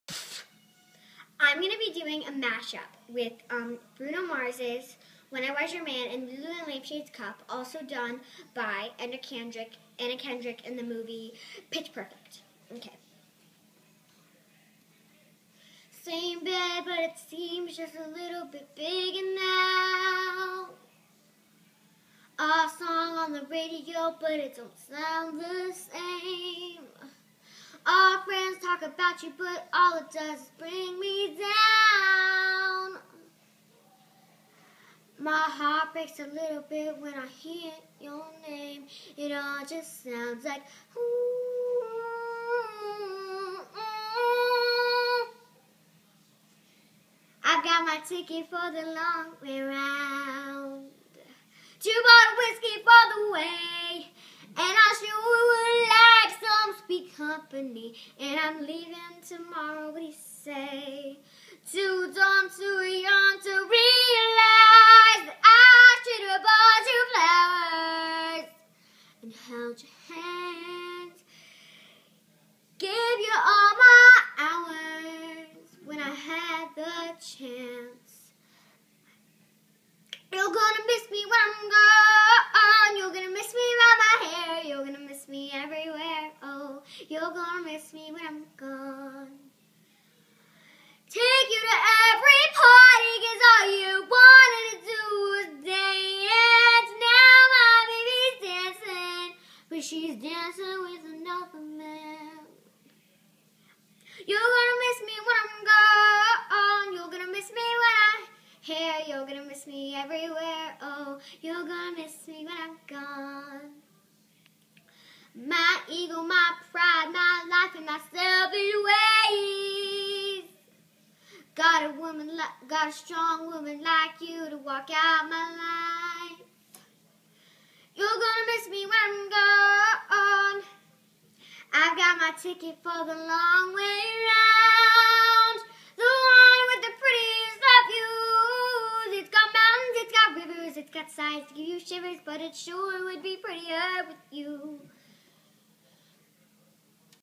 Small Fry's Mashup
Bruno Mars and Lulu and the Lampshade mashed into one.